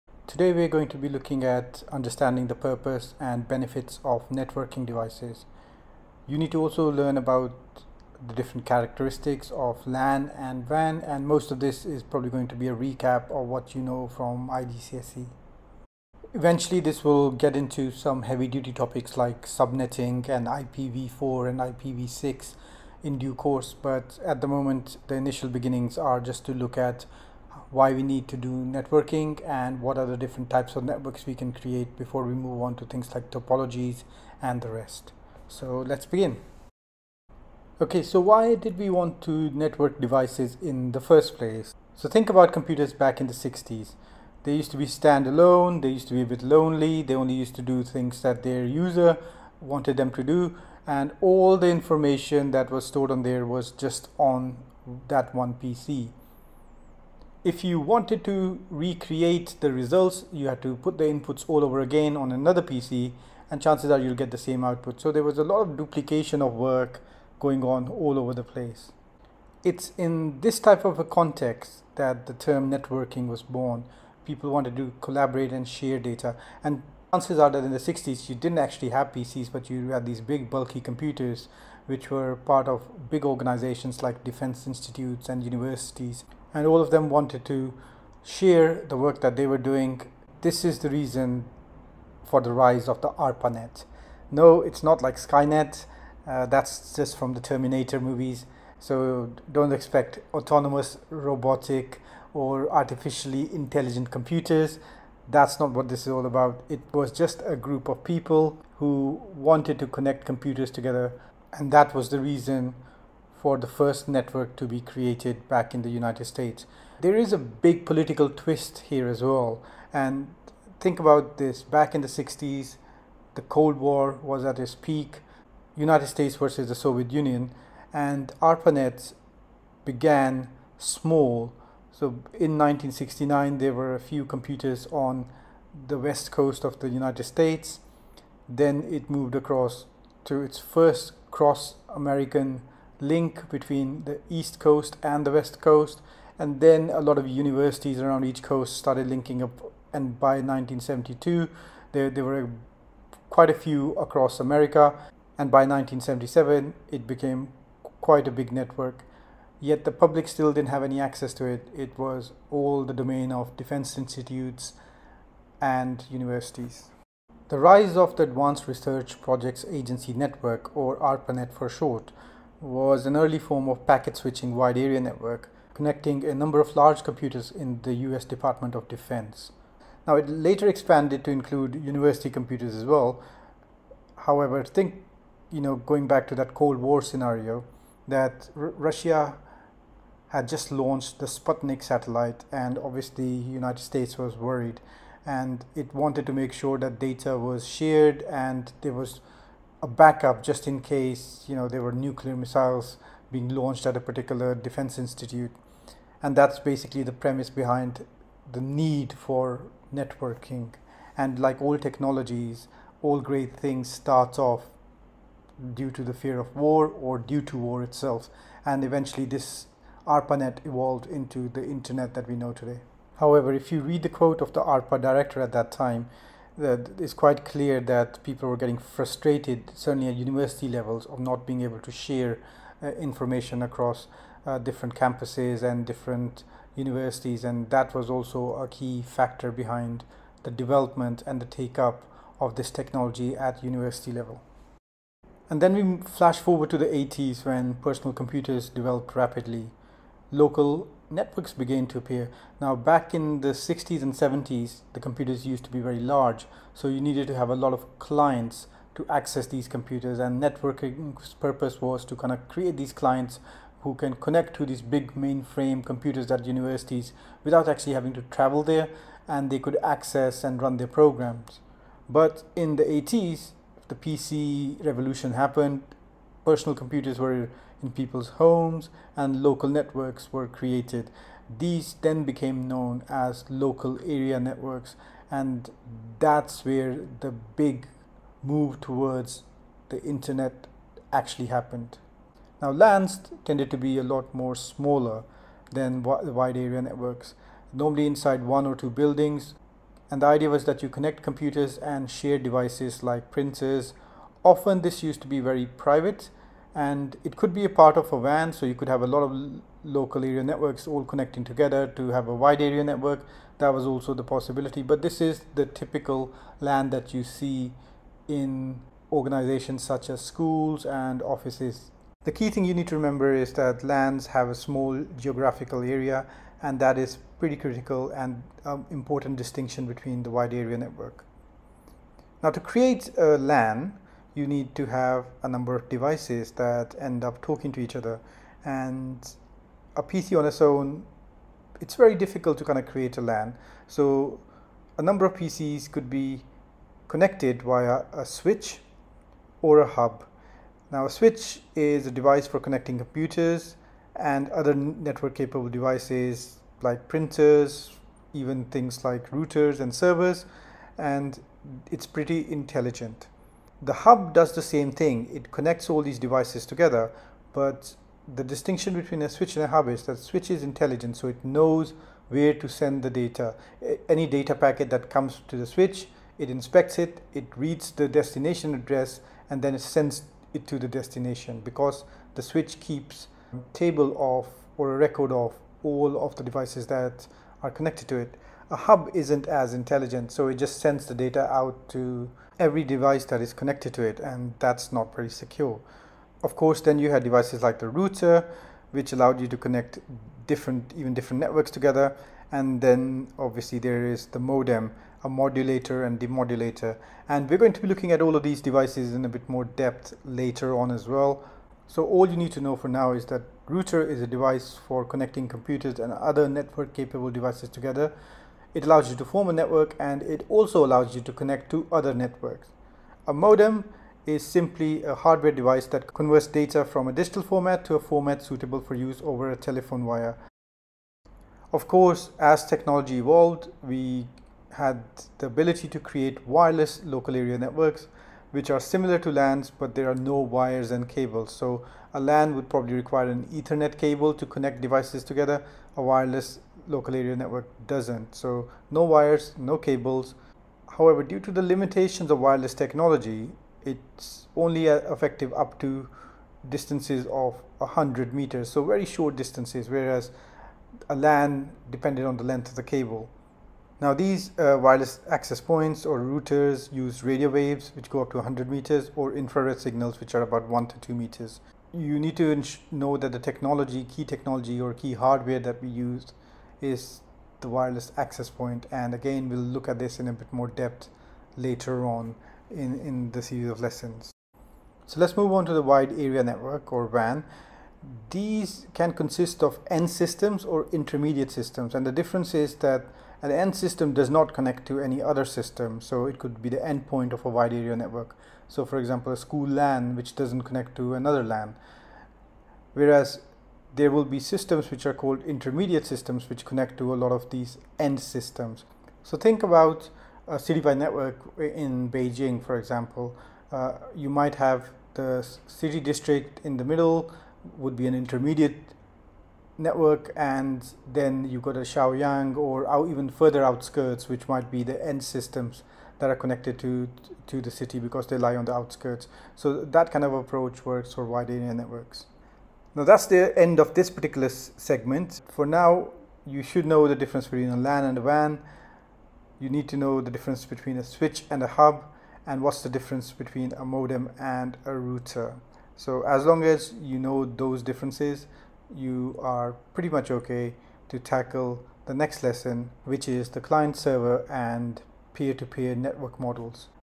Lesson 8 - Networks introduction
Lesson 8 of the AS level Computer Science course introduces the topic networks and focusses on LAN, WLAN and WAN